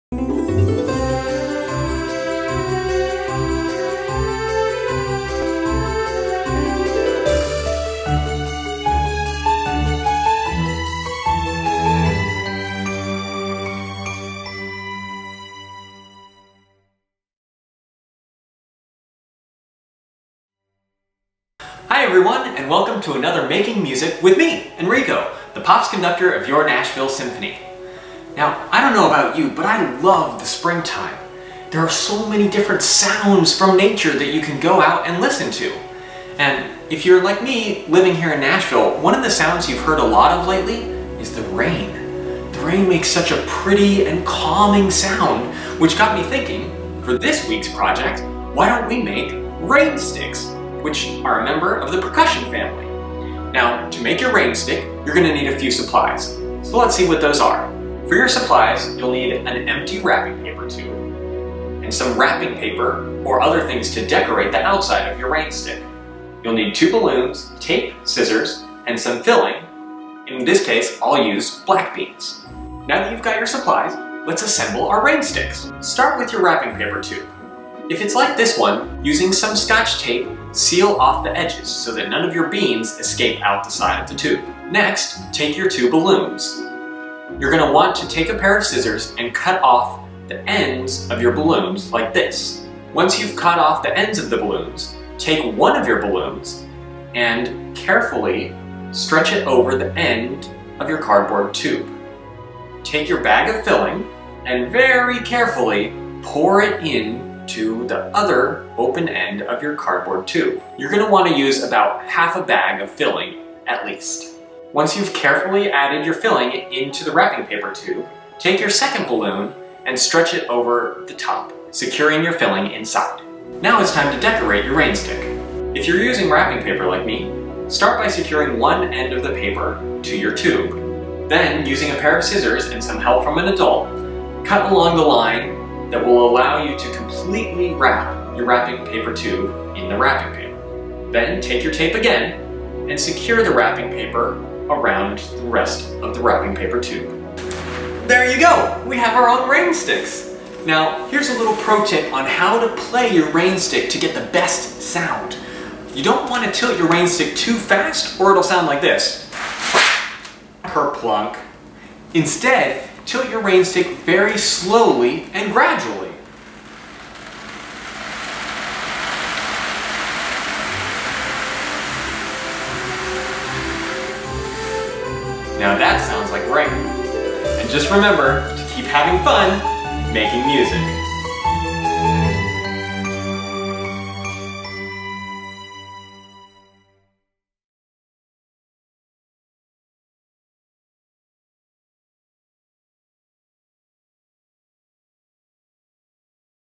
Making Music Enrico: Rainstick
In this week's edition of Making Music with Enrico Lopez-Yañez, learn how to make a rainstick at home.

View past episodes here: NashvilleSymphony.org/homeresources

Brought to you in part by Nissan, Dollar General, the Ann & Monroe Carell Foundation and AB.